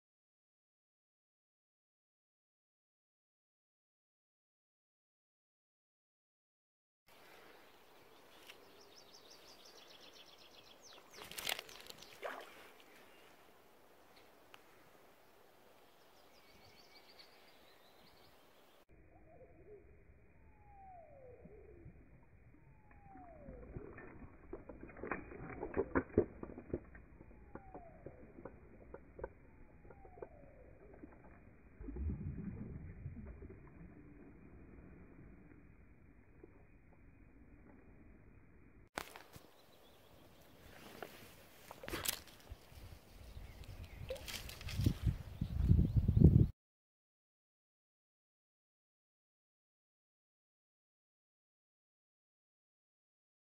Fishing the Swedish Mayfly on June the 6th
Short movie showing trouts going for my, "Swedish Mayfly". (my new pattern) ;-)